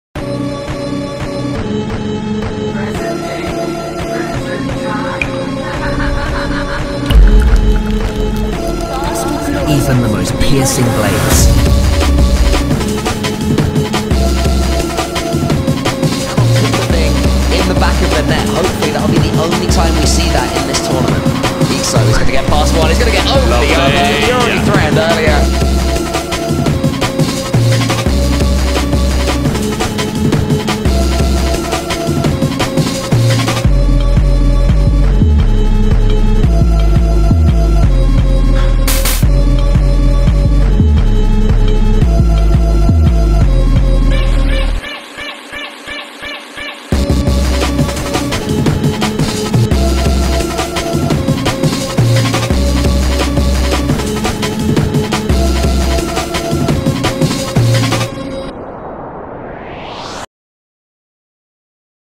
ANIMATION SHOWREEL
Thought I'd show you guys some of my favourite projects from the last year or two. Some of which I did not have the rights to share fully on the channel. Hope you enjoy :)